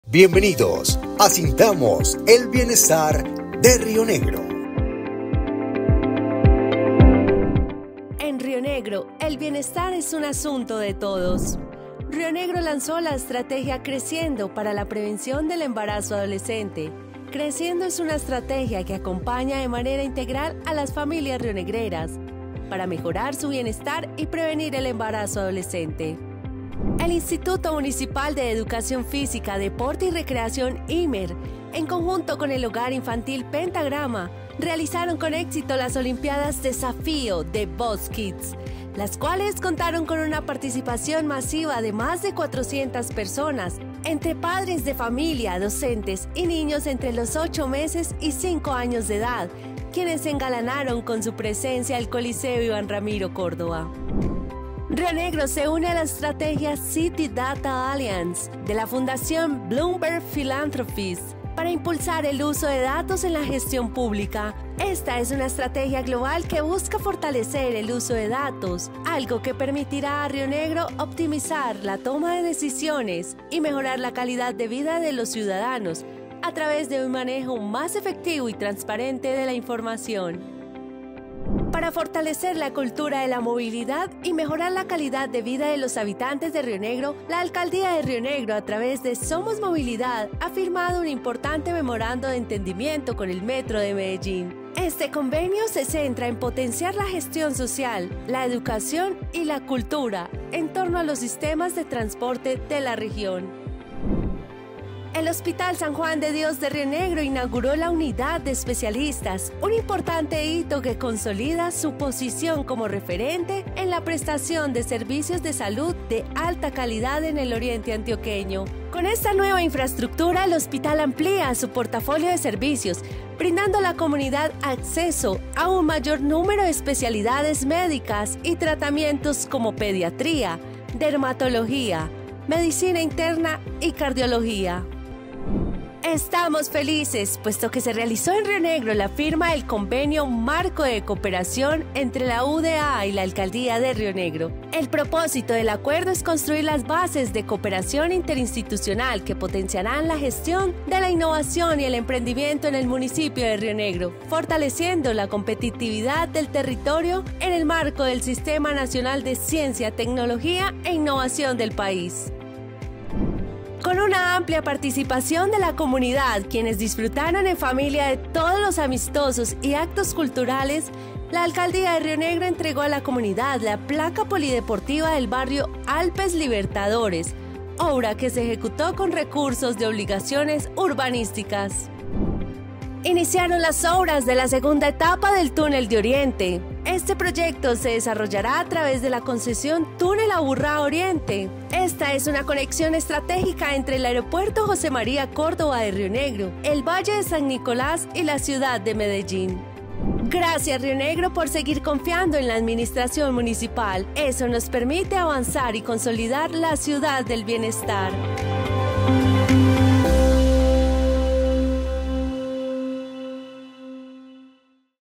Sintamos el Bienestar de Rionegro ❤️
Aquí el bienestar es un asuntos de todos, por eso queremos contarte nuestros logros más significativos durante el mes de Agosto 🙌🏼